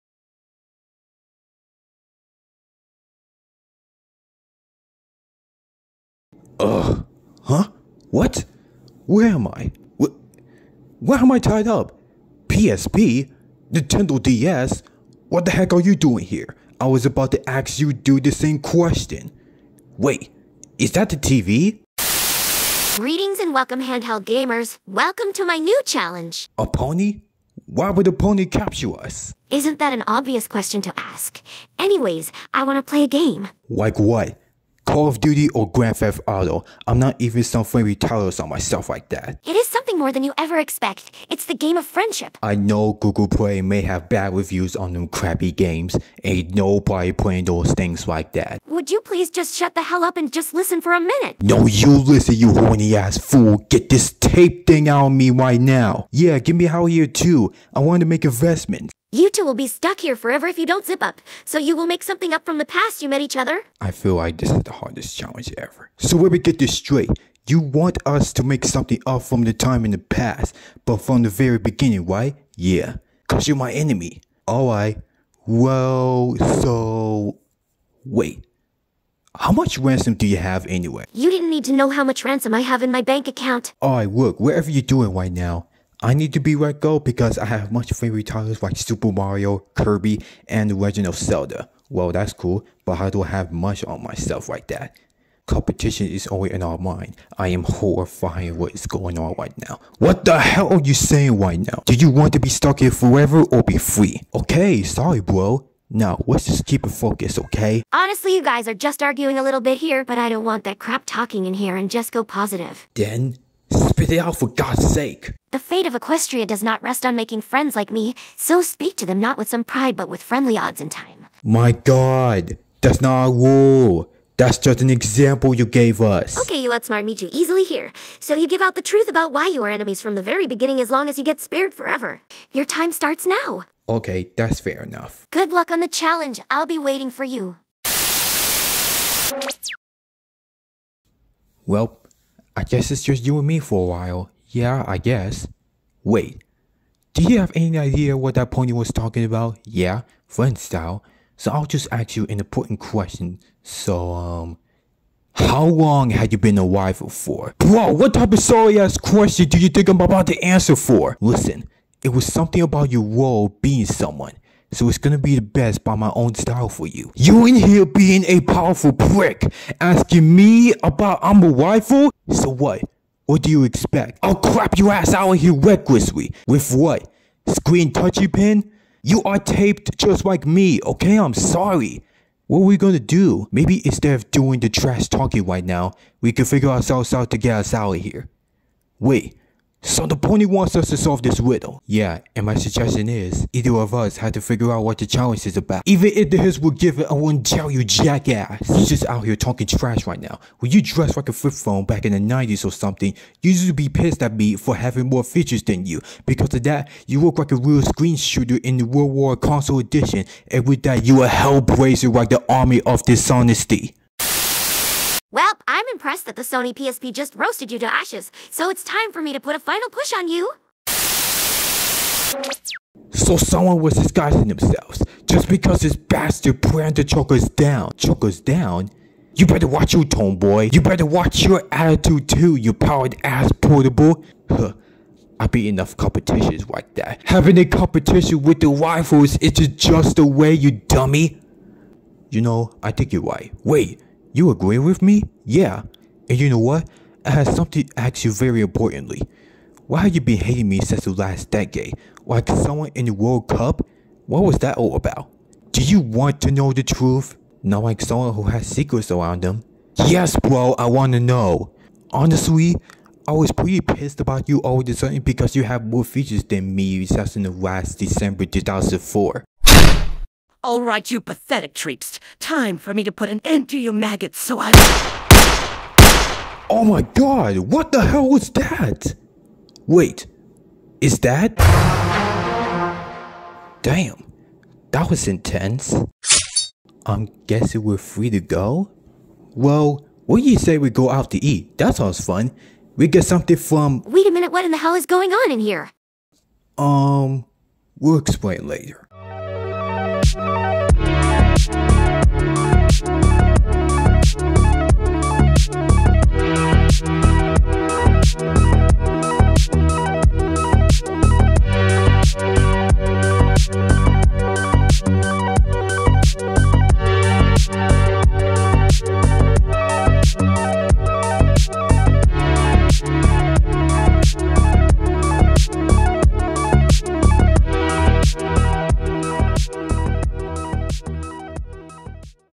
Life With Animation Episode 26 - Rivals (Handheld Consoles)
Sony PSP and Nintendo DS are trapped, by a disguisable genius from Episode 7 of this series (again). Can this happen to them forever? Find out here...

Sub to Sethical. I won't be waiting much longer.

Twilight Sparkle Voice used with 15.ai
Green Screen by GreenScreenPonies

Remember, I don't associate with ripoff talk.